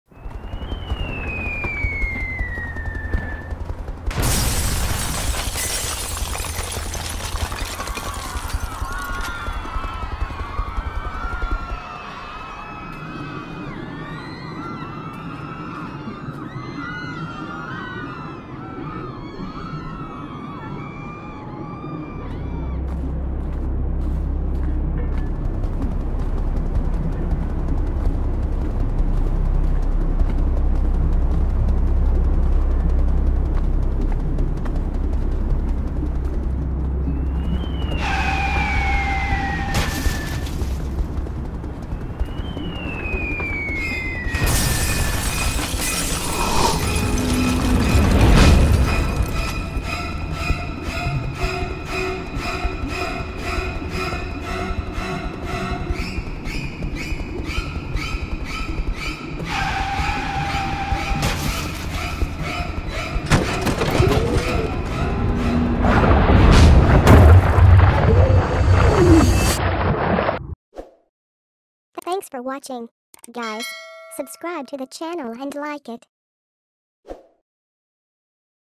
Giant Shark in the City Center: How Is This Possible? -360º/VR video
As the city goes about its daily routine, a bizarre and terrifying sight emerges—a gigantic shark appears, swimming as if it's in water, right in the middle of the bustling streets. People are in shock and disbelief, capturing the surreal event on their phones. How did this massive sea creature end up here, and what does it mean for the city? Witness the astonishing spectacle as the mystery unfolds!